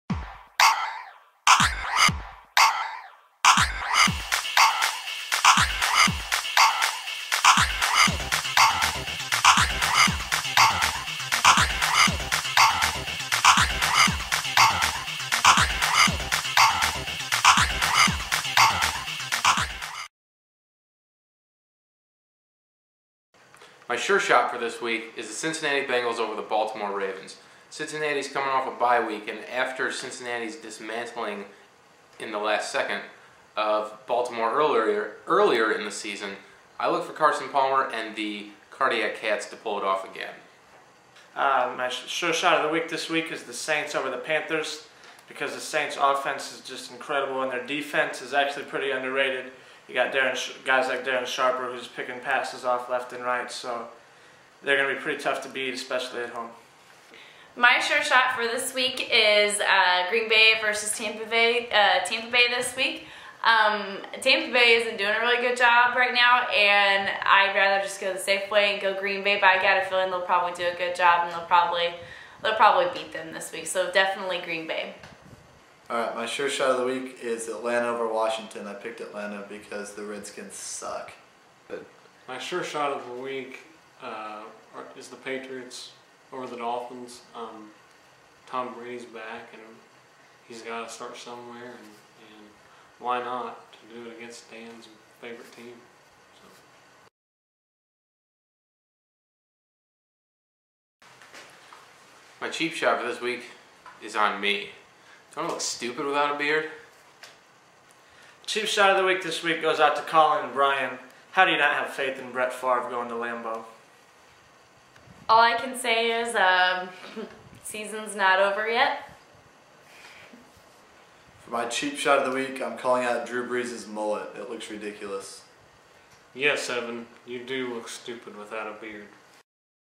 Pigskin Picks Week 8
Franklin College football experts make their pics for week 8 in the NFL